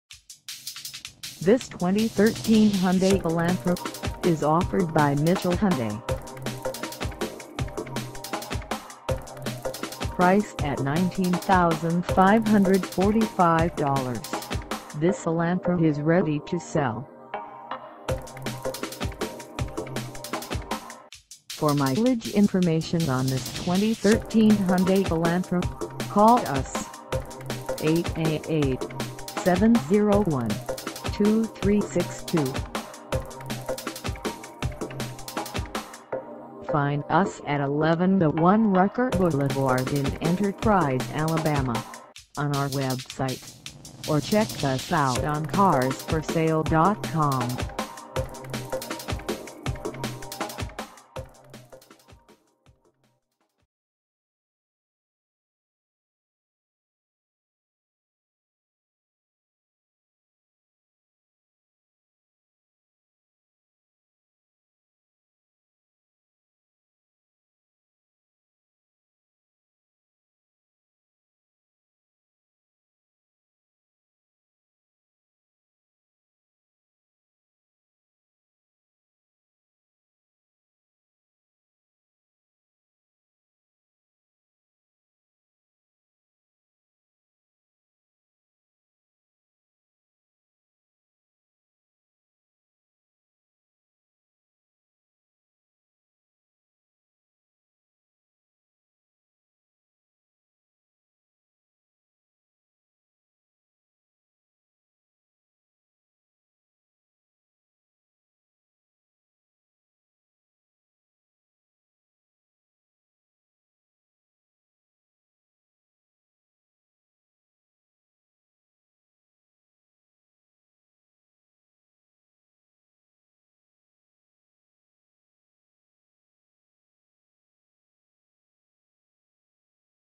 2013 Hyundai Elantra GLS - for sale in ENTERPRISE, AL 36330
MITCHELL HYUNDAI
1101 RUCKER BLVD

in ENTERPRISE, AL 36330

Come test dirve this 2013 Hyundai Elantra GLS for sale in ENTERPRISE, AL.